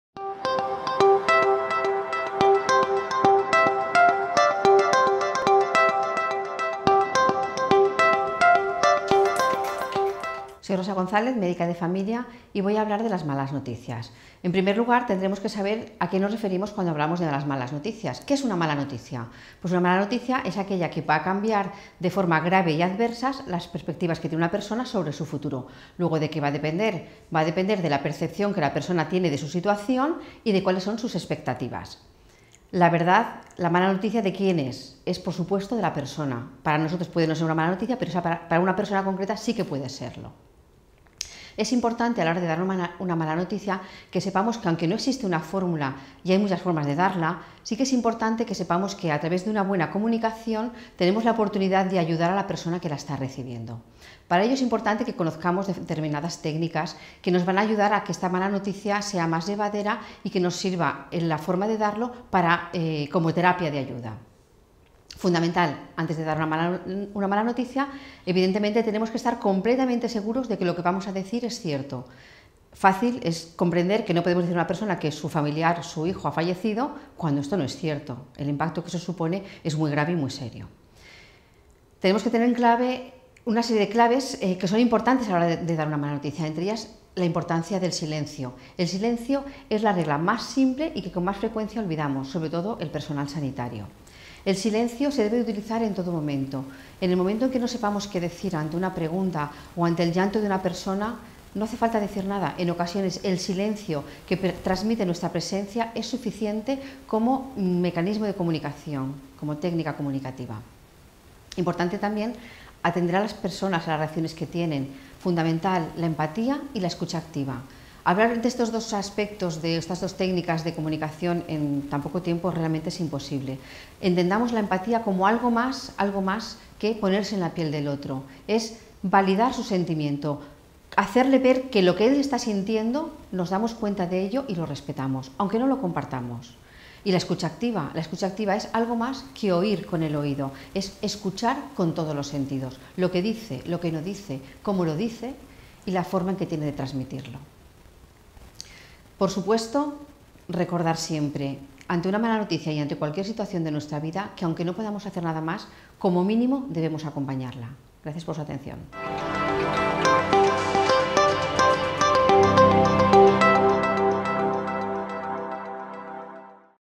11.2. Malas noticias
Vídeo correspondiente a la 2ª lección del Módulo 10: "Atención domiciliaria y cuidados paliativos" del MOOC de "Medicina familiar y atención primaria"